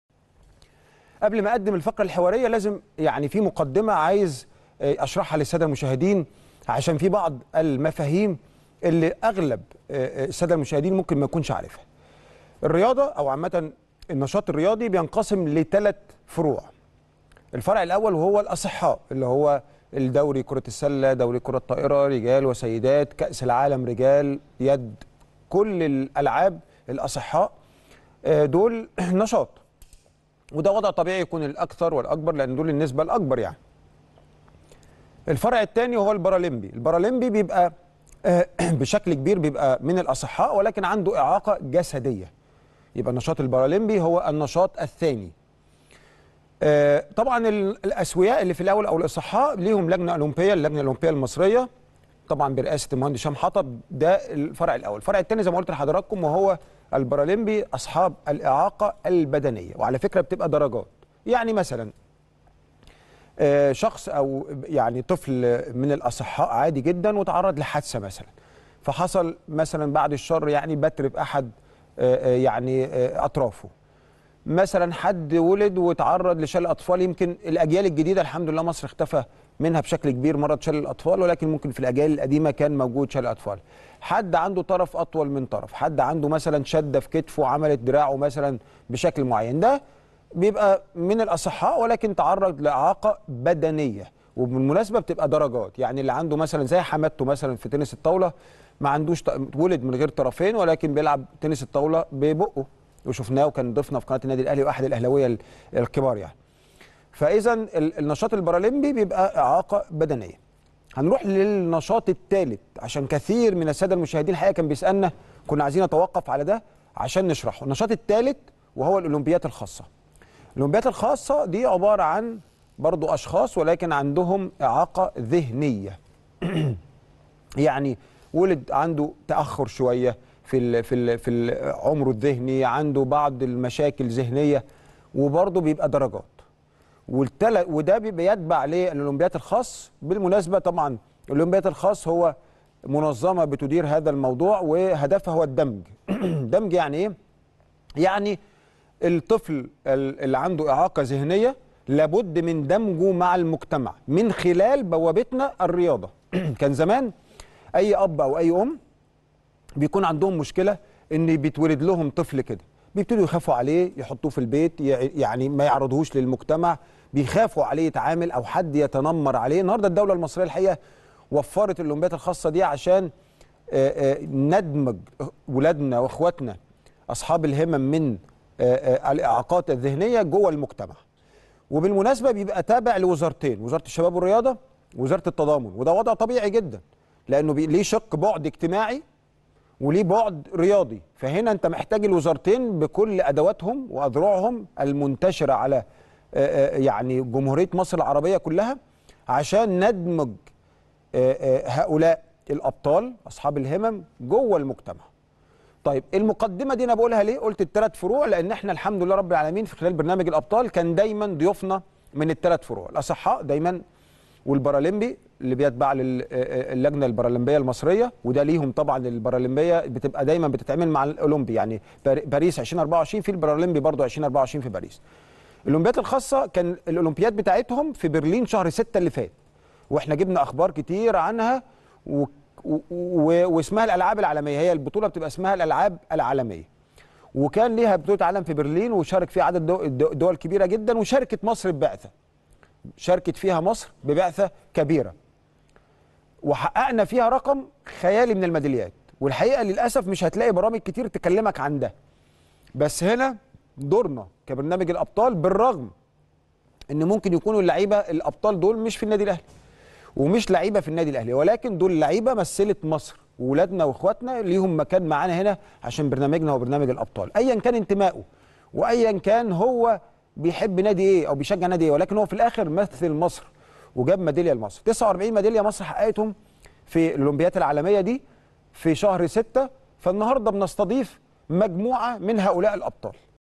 هيثم السعيد يوضح مفاهيم النشاط الرياضي .. والفرق بين " الأولمبياد الخاصة والألعاب البارلمبية " 🧑🏻‍🦽
الكابتن هيثم السعيد يوضح مفاهيم وأقسام النشاط الرياضي .. والفرق بين " الأولمبياد الخاصة والألعاب البارلمبية " 🧑🏻‍🦽🧑🏽‍🦼